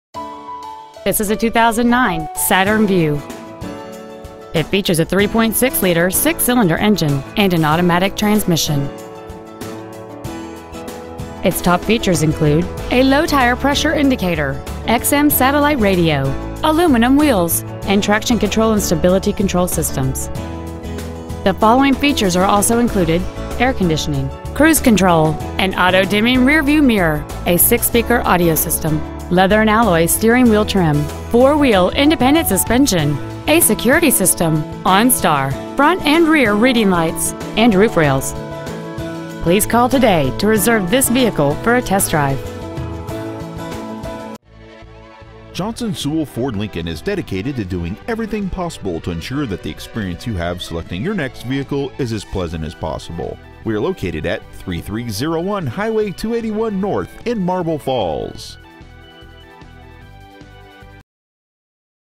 2009 Saturn VUE Marble Falls TX
Year : 2009
 Make : Saturn
 Model : VUE
 Engine : 3.6L V6 24V MPFI DOHC
 Trans . : 6-SPEED AUTOMATIC
 Exterior : SILVER
 Miles : 70,744
 Interior : GRAY
 Stock : PWW9867